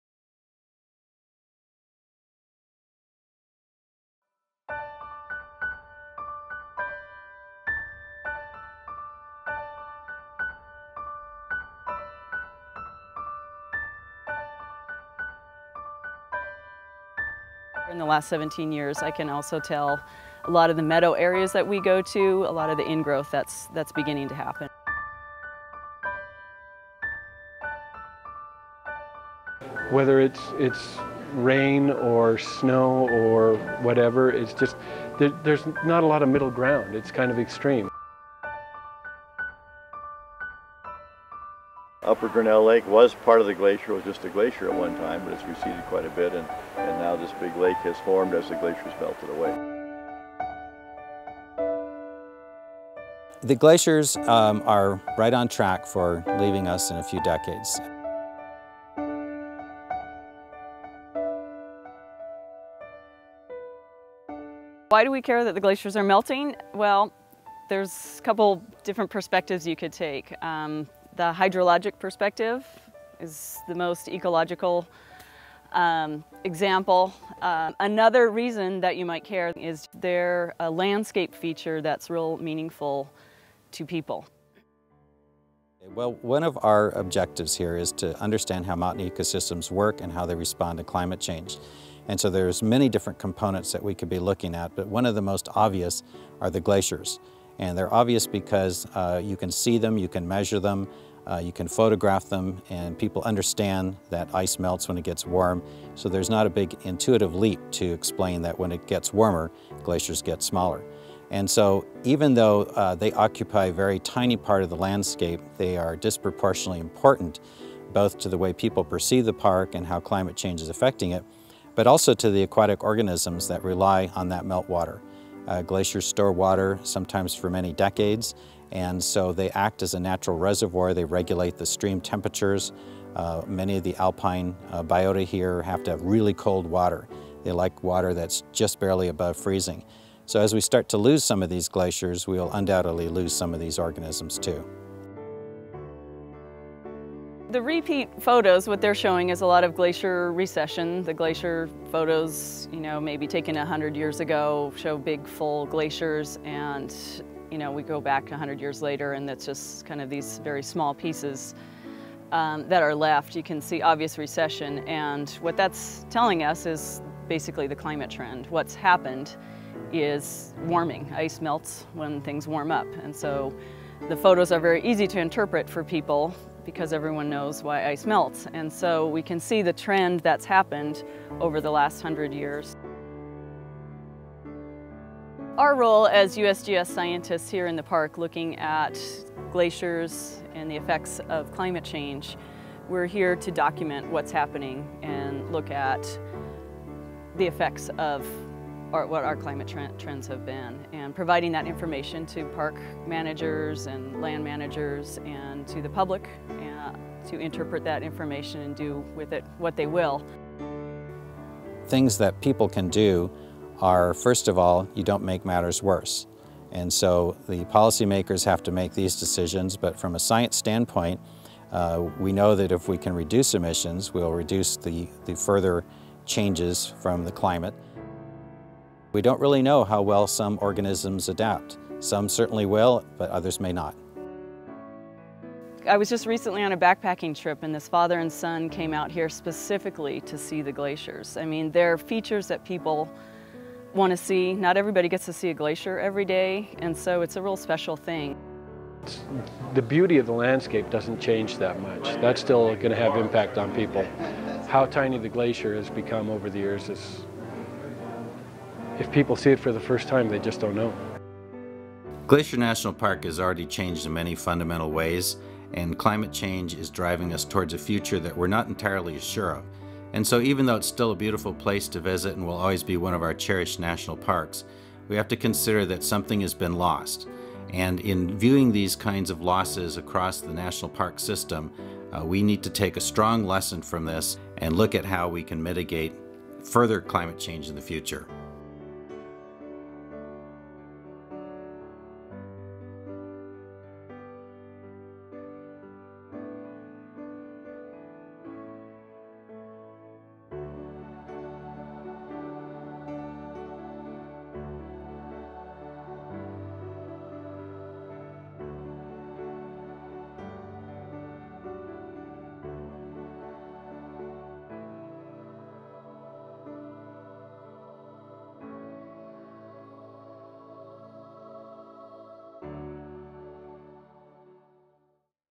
A Changing Landscape
This video is about how climate change is affecting Glacier National Park.